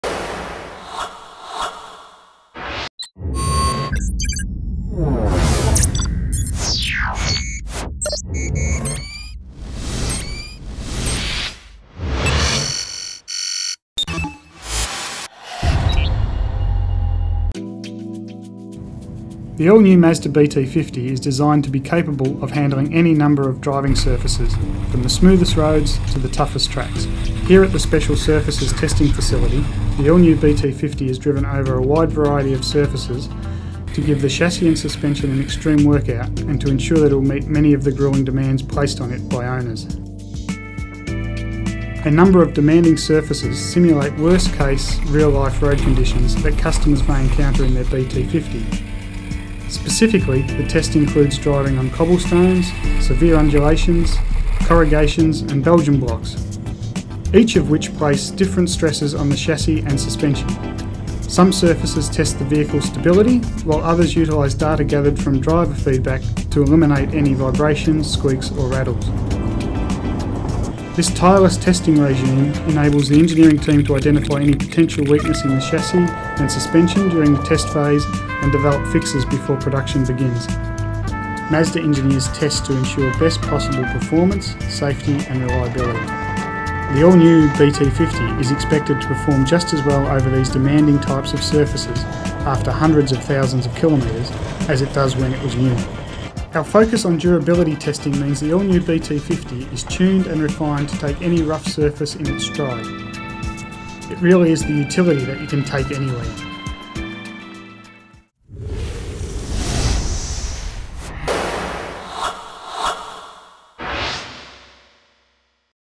Mazda BT-50 Special Surfaces Testing
Watch as Mazda puts it's BT-50 through some seriously testing road and driving conditions to ensure all components are up to the challenge, and are working to Mazda's high standard.